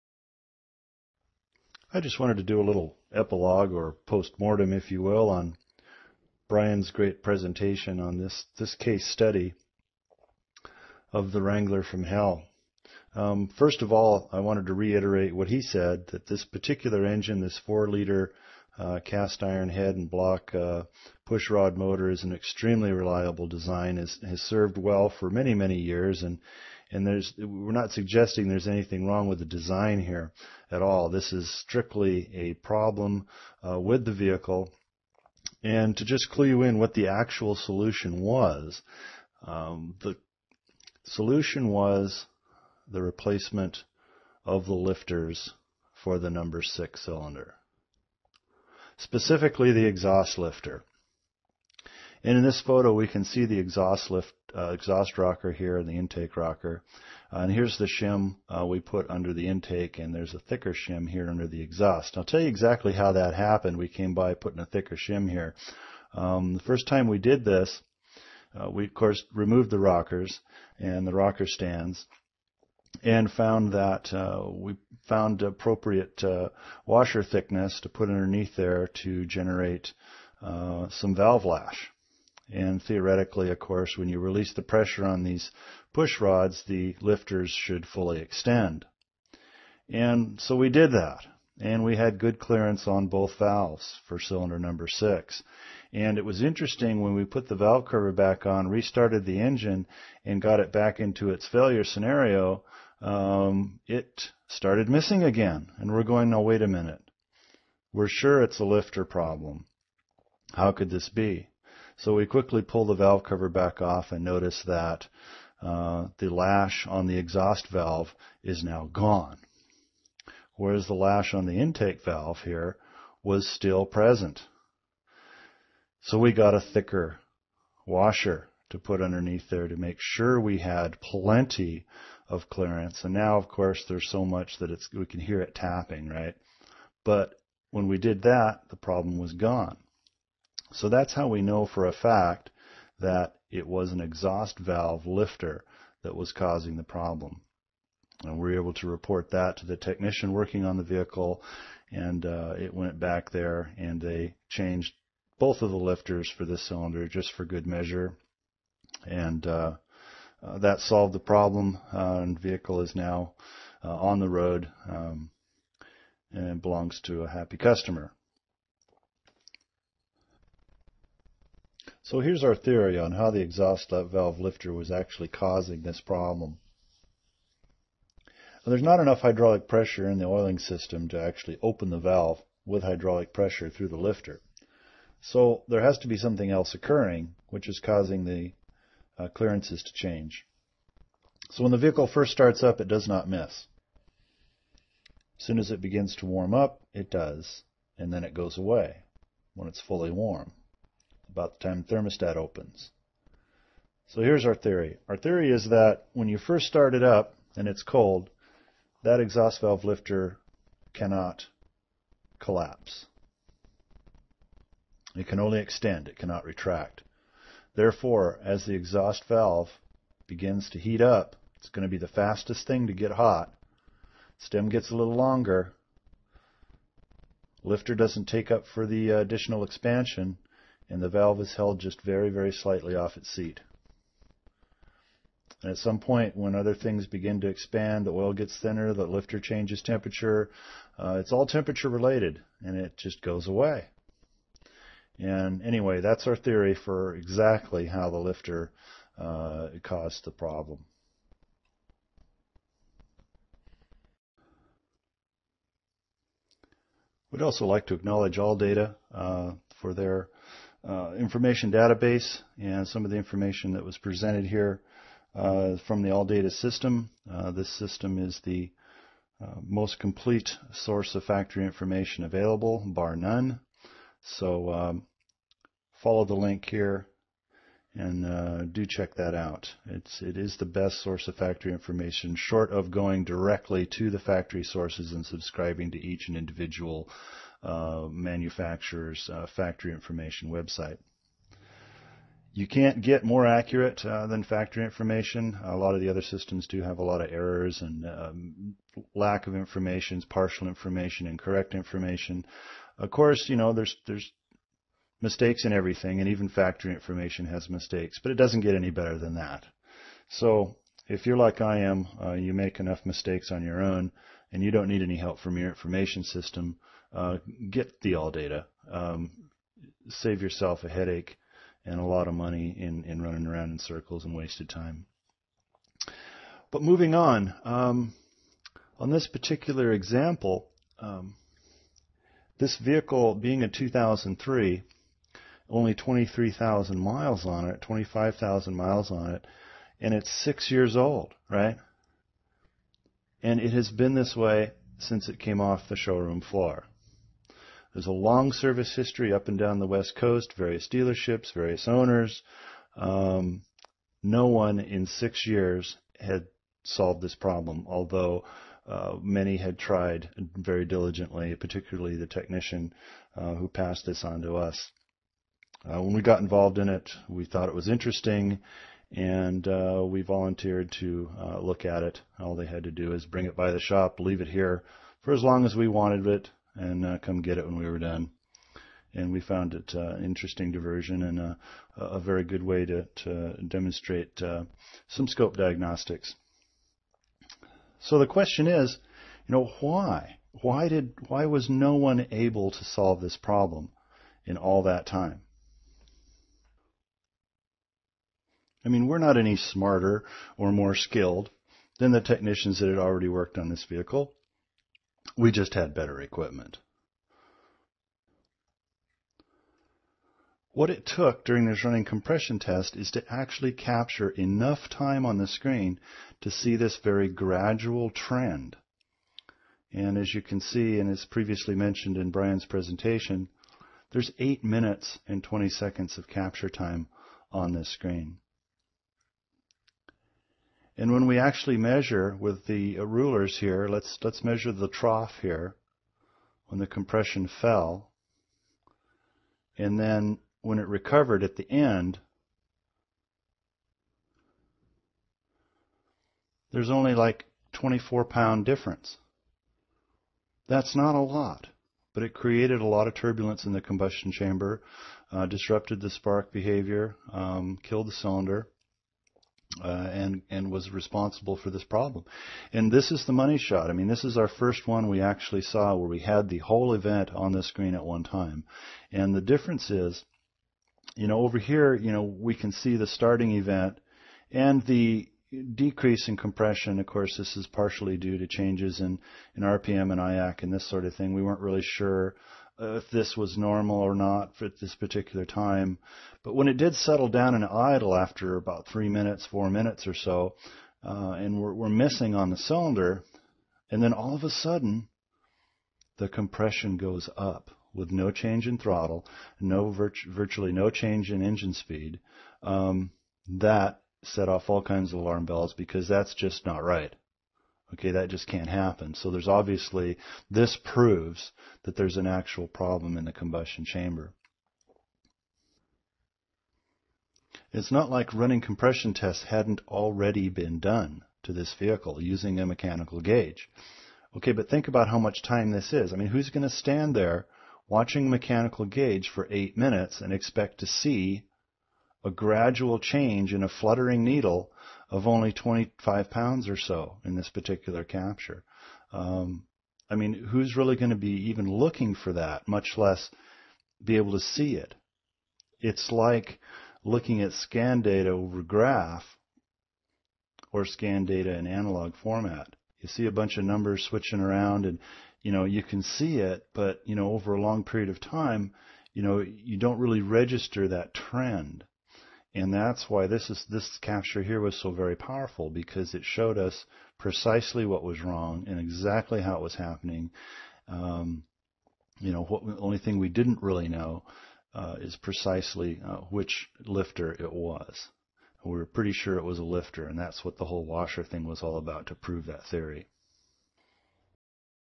The Wrangler from Hell part 2
The unfixable fixed with the power of the PicoScope. Part 2